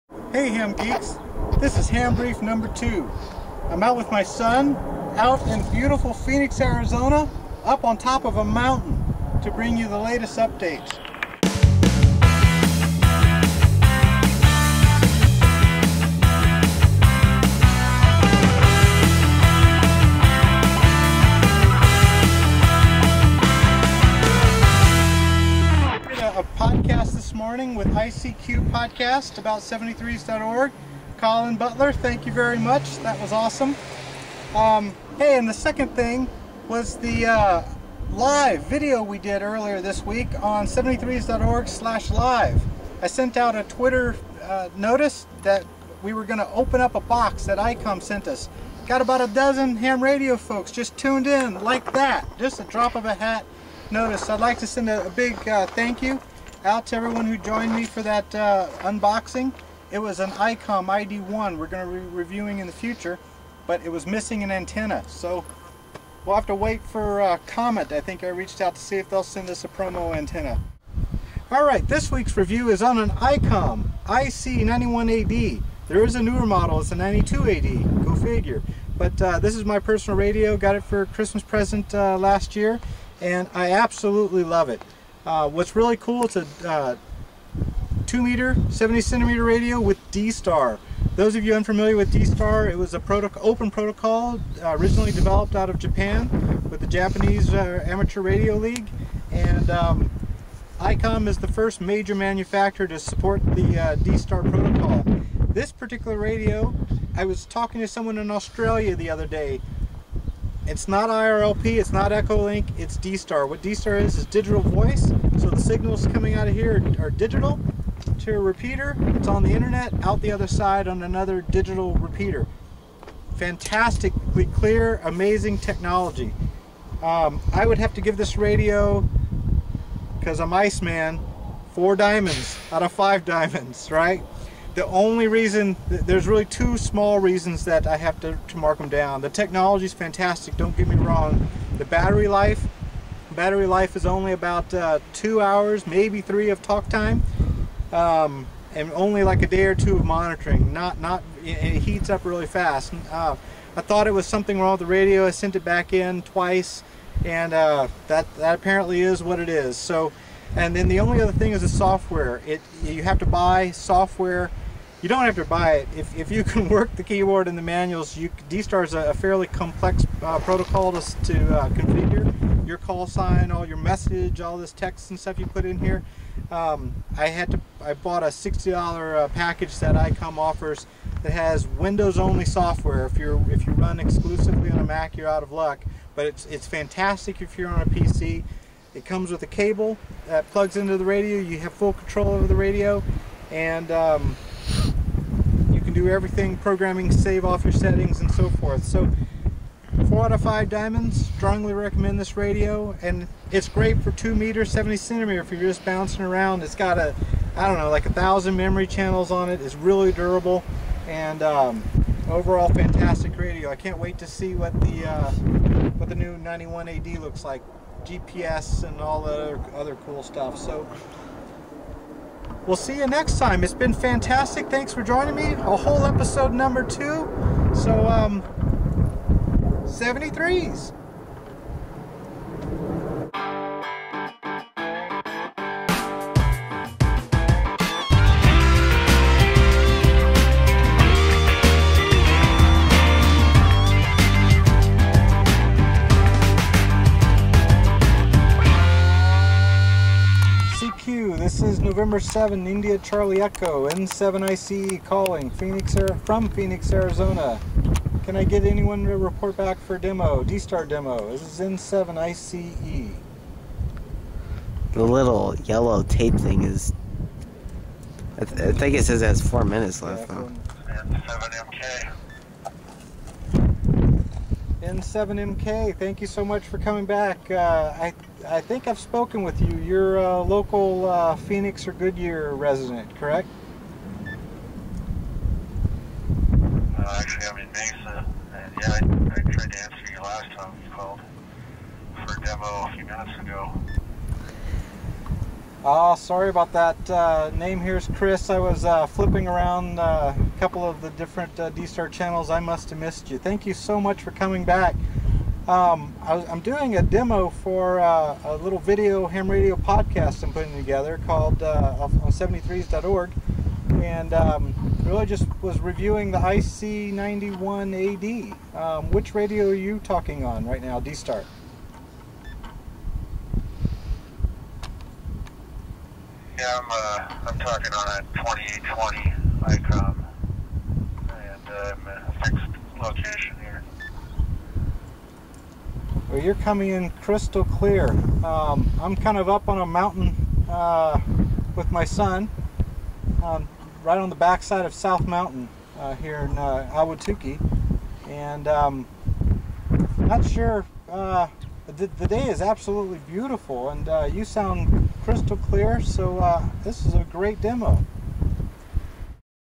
73s.org HamBrief 2
Chris Matthieu, N7ICE, reviews the Icom IC-91AD DStar ham radio